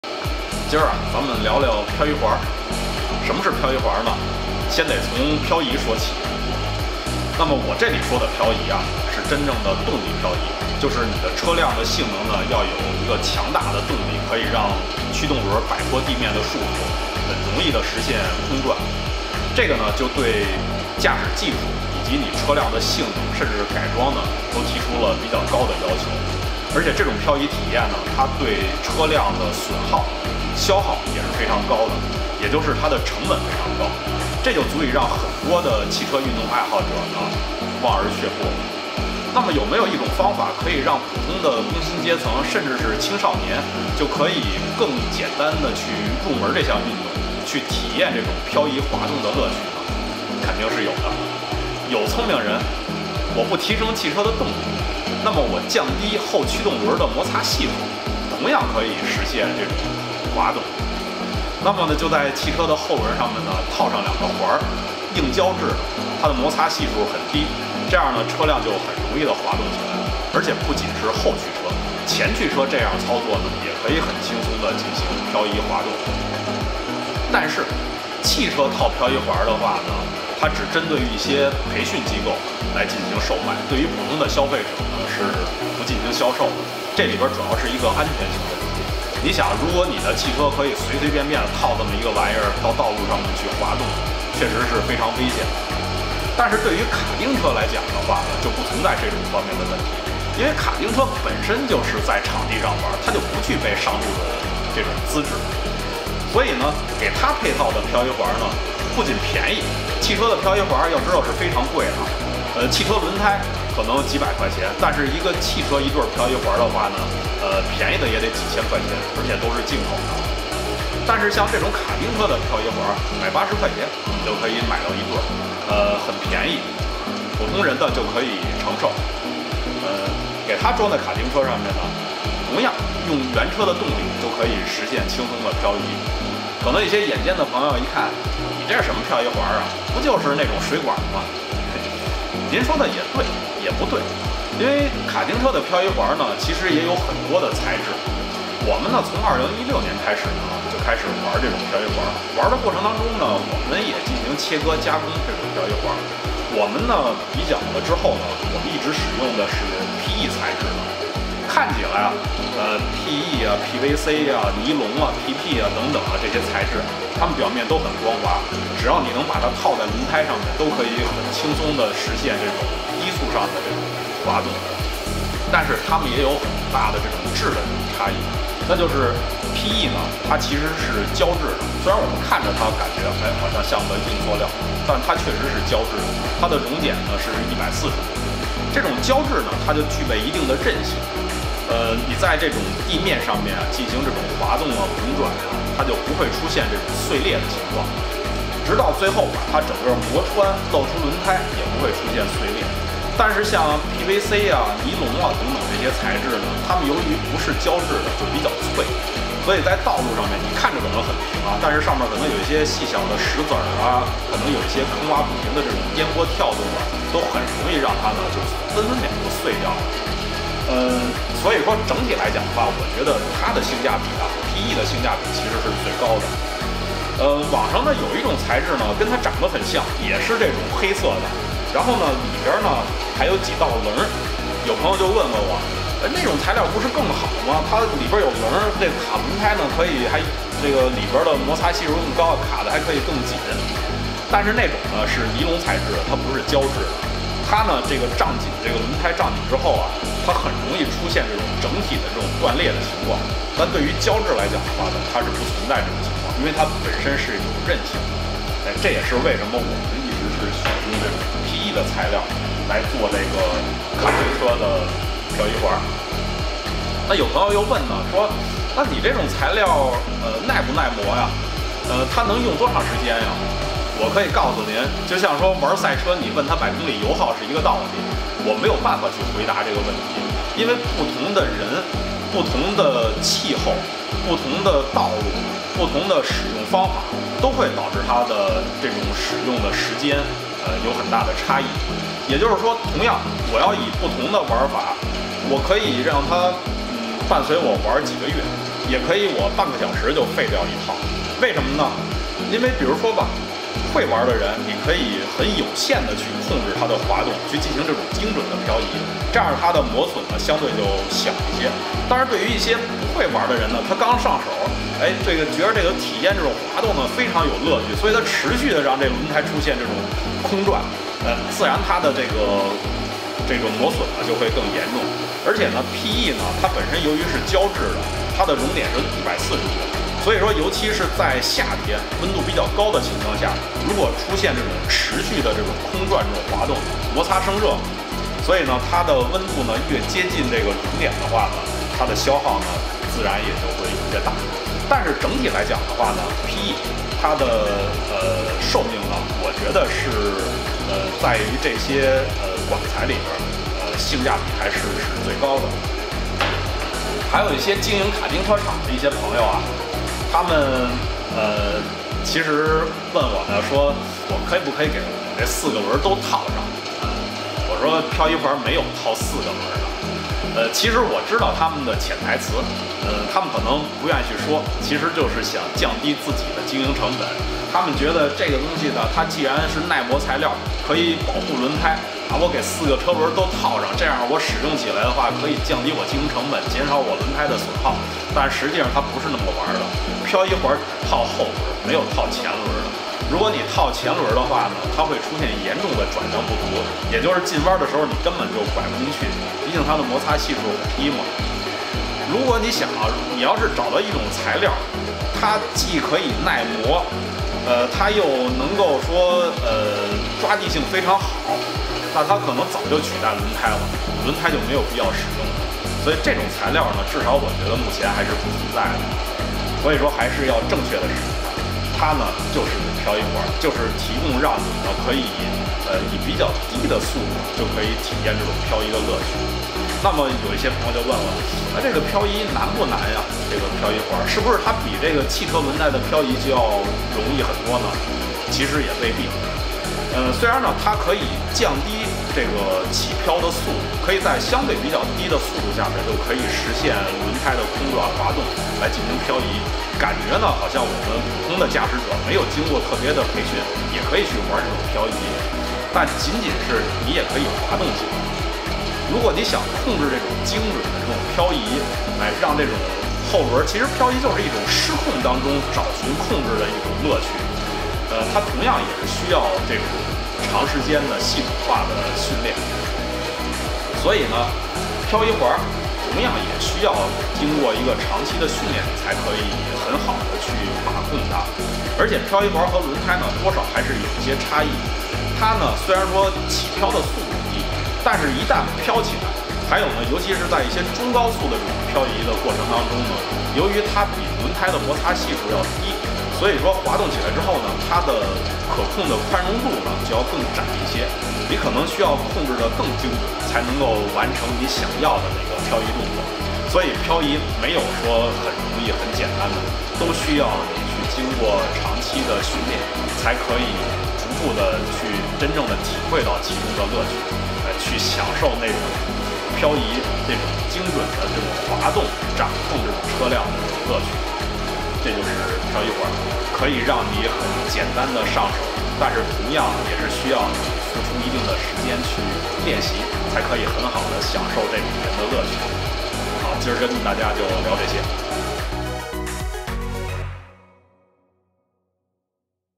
简单容易上手的卡丁车PE漂移环到底是啥来头
聊聊卡丁车的漂移环，看似简单的物件其实里面还是有很多学问和名堂的，花几分钟搞懂，你就可以简单从容的上手体验快乐漂移了！